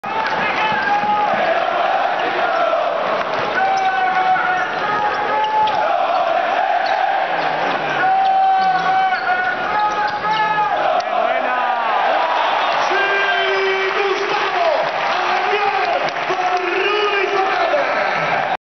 Rudy & Ayon
Bonito Alley Op entre Rudy Fernandez y el mate de Gustavo Ayon